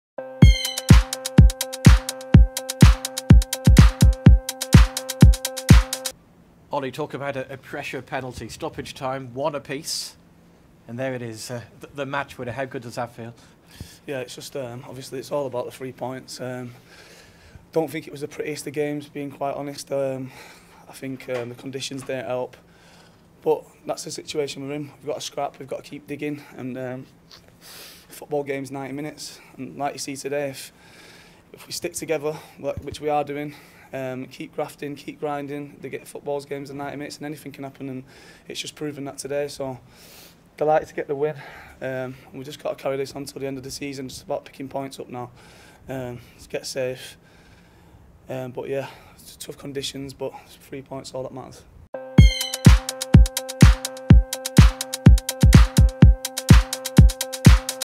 INTERVIEW: Ollie Norburn Post Wycombe Wanderers - Town TV
Ollie Norburn reflects on Town's 2-1 victory over Wycombe Wanderers- full interview available on IFollowShrews